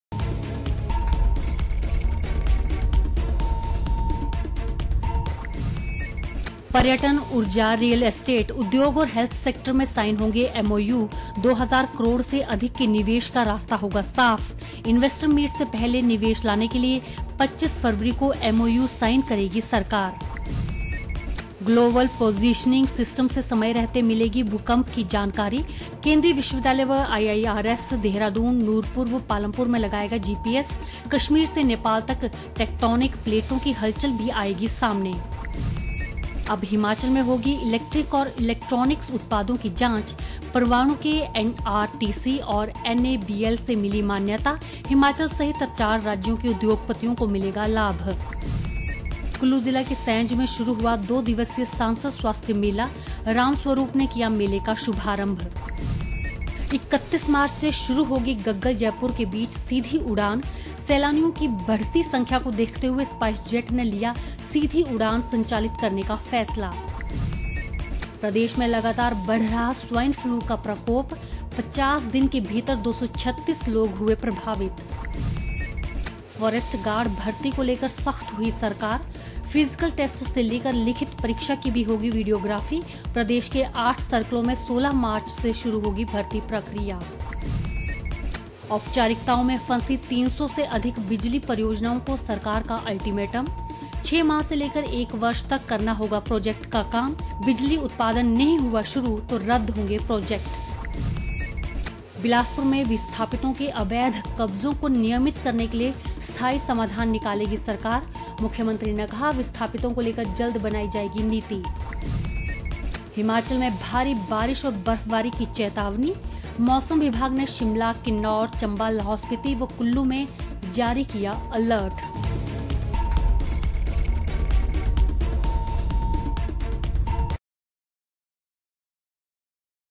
हिमाचल-फटाफट खबरें 20 /02 /2019
हिमाचल में भारी वर्षा व बर्फ़बारी की चेतावनी- अन्य खबरों के लिए देखिये- हिमाचल फटाफट खबरें 20 /02 /19